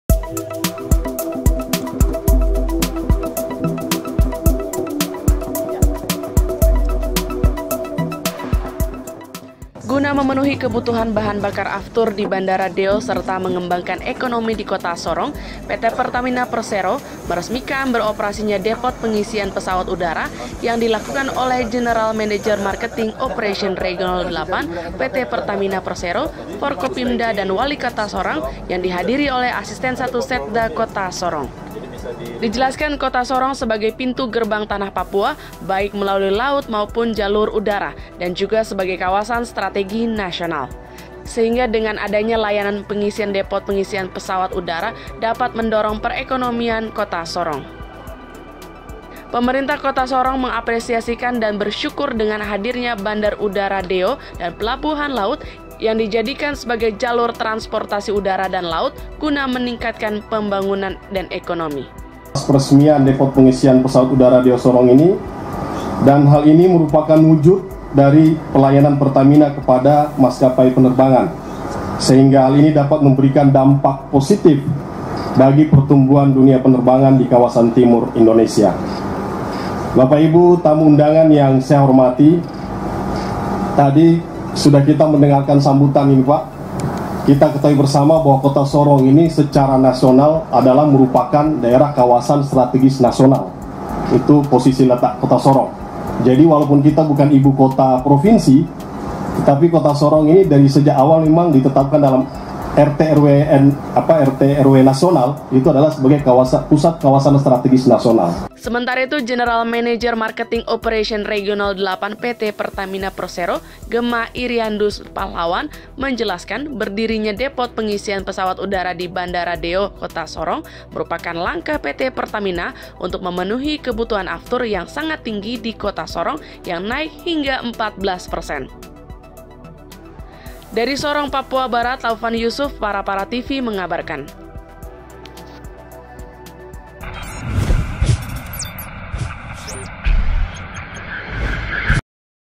PERESMIAN DPPU BANDARA DEO KOTA SORONG
Paraparatv.id | Sorong Kota | Guna memenuhi kebutuhan bahan bakar avthur di Bandara Deo serta pengembangan ekonomi di kota Sorong,  PT Pertamina Persero , meresmikan depot pengisian pesawat udara yang dilakukan oleh General Manager Marketing Opration Regional VIII PT Pertamina Persero, Forkopimda dan Wali Kota Sorong Yang dihadiri Asisten 1 Setda Kota Sorong.

Kota Sorong, sebagai pintu gerbang Tanah Papua, baik melalui laut, maupun jalur udara dan juga sebagai kawasan strategi Nasional.  Sehingga dengan adanya layanan pengisian depot pengisian pesawat udara dapat mendorong perekonomian kota Sorong.

Pemerintah Kota Sorong mengapresiasi dan bersyukur dengan hadirnya bandara Deo dan Pelabuhan Laut yang dijadikan sebagai jalur transportasi udara dan laut/ guna meningkatkan Pebangunan dan ekonomi.

Sementara itu,  General Manager Marketing Opration Regional viii Pt. Pertamina persero, Gemah Iriandus Pahlawan, menjelaskan berdirinya depot pengisian Pesawat Udara Di Bandara Deo Kota Sorong, merupakan langkah PT Pertamina  untuk memenuhi kebutuhan avthur yang sangat tinggi di Kota Sorong, yang naik hingga 14 persen.

Laporan : Taufan Yusuf